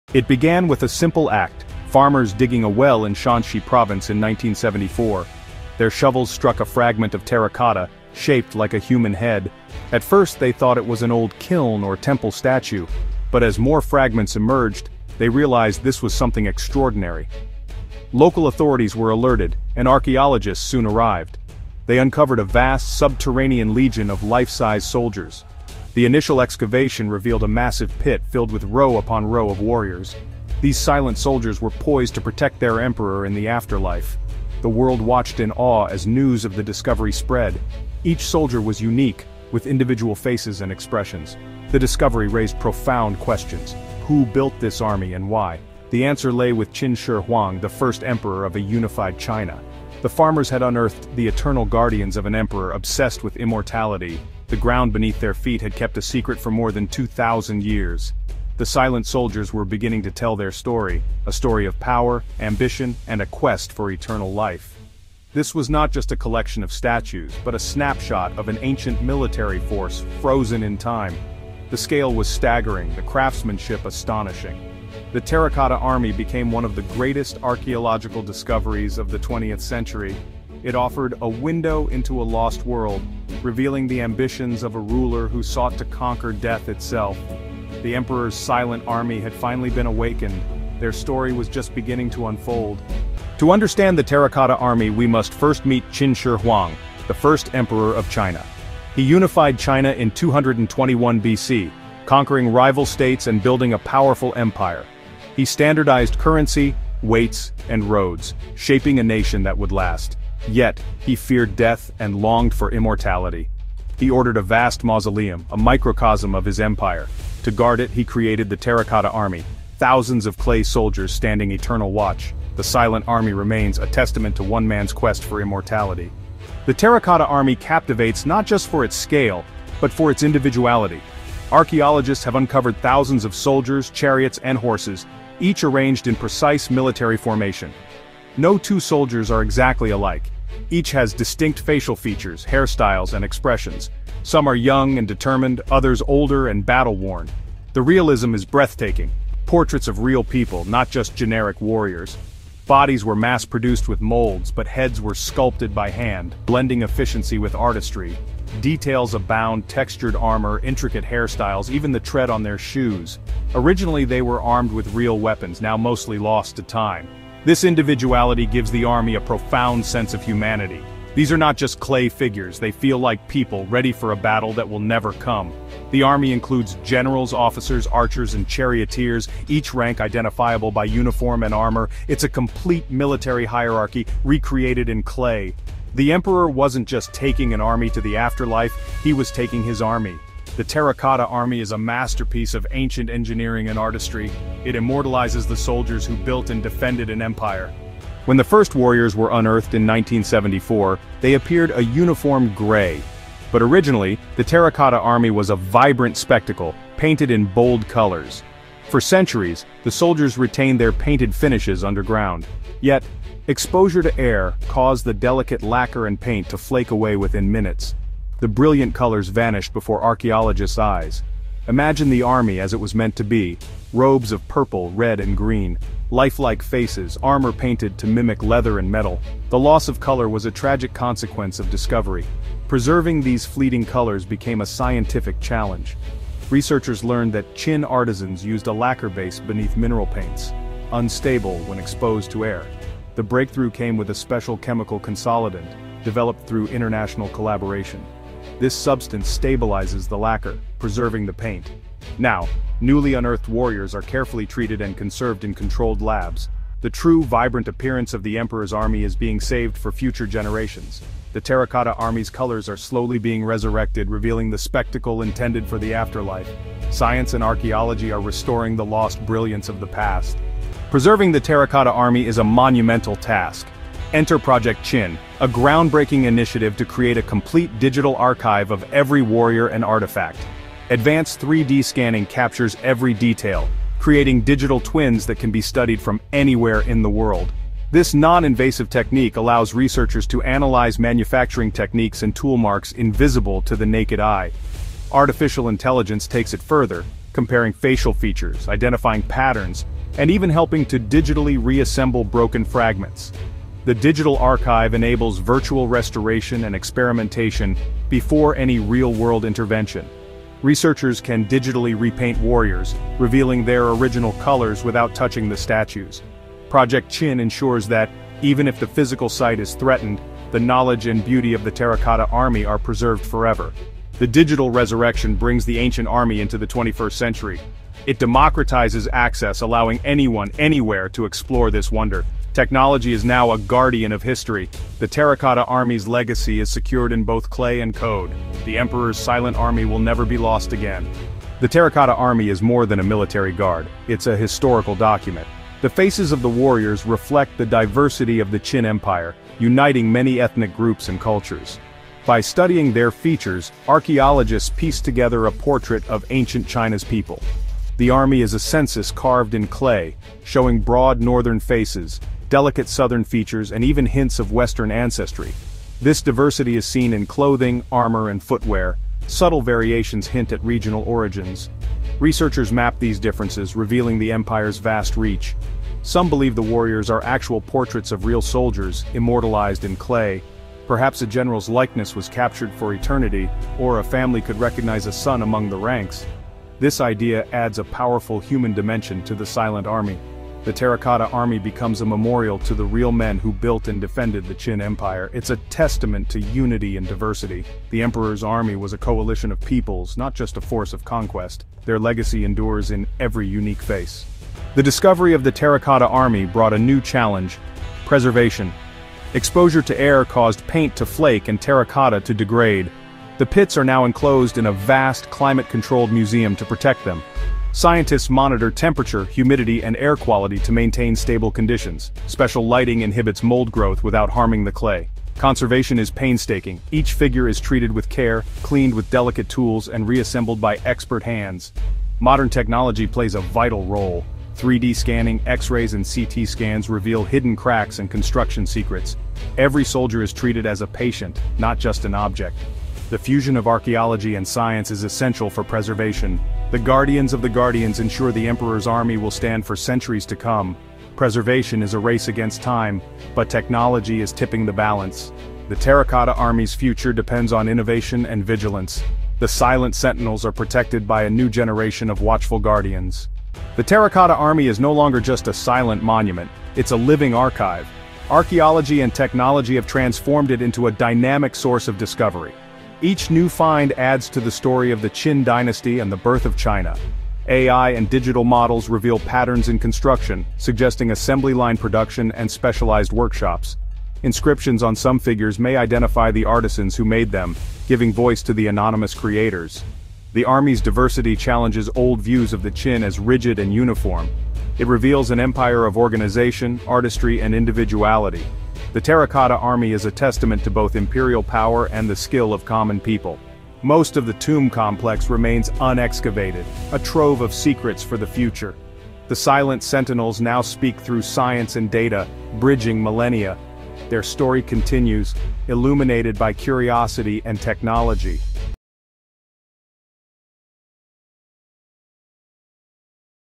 Terracotta Army Scanned By AI In 2025 — And What It Exposed Will Stun You!
In 1974, farmers in China uncovered the legendary Terracotta Army, a vast underground force built to guard Emperor Qin Shi Huang in the afterlife. Thousands of life-sized warriors stood in battle formation, each with unique features, revealing astonishing individuality. Centuries-old secrets began to emerge, showing not only artistry but also the empire’s diversity. Modern AI and 3D scanning unlocked new insights, proving these were portraits of real people from across the Qin Empire. Today, technology preserves their legacy, ensuring the Silent Army’s story continues to inspire the world.